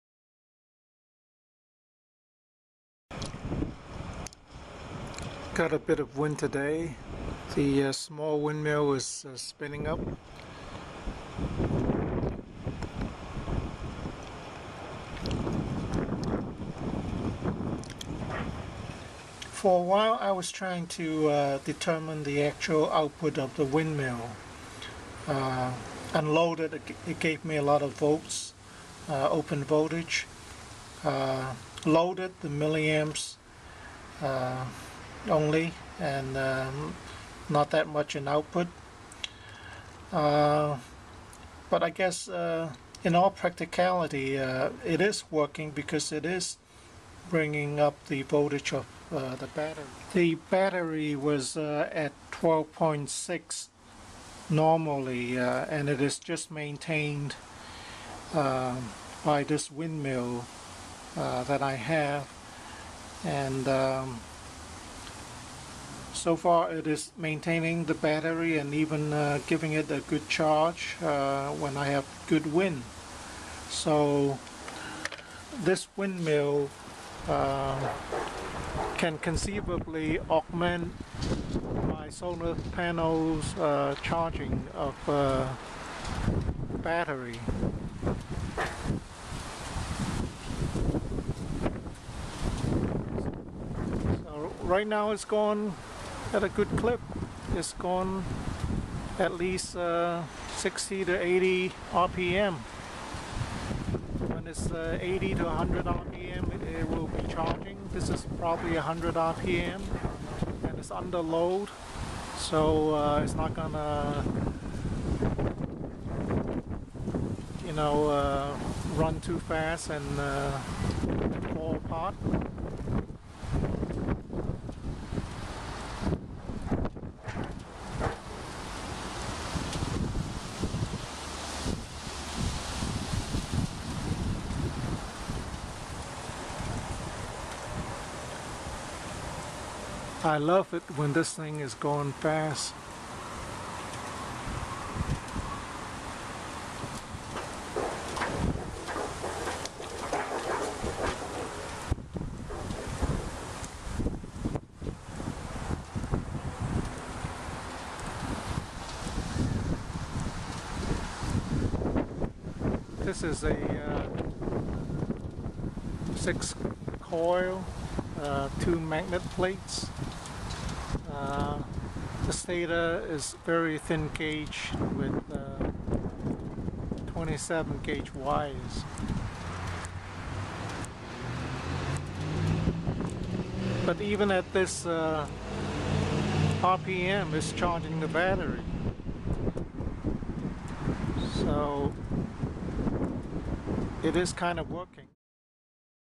Vertical wind turbine charging battery.mp4
I have been wondering how well this windmill would work in real world situations. With the small output in ma, I'm not sure if it's worthwhile to have it at all. Today is a windy day and I have the answer. My small 18 amp hour battery is usually at 12.6v.  When I came back from shopping, the voltage went up to 12.72v. Not much but it is a definite improvement. The windmill has a bit of rattling in high winds. Luckily, It is standing up and has not fly apart.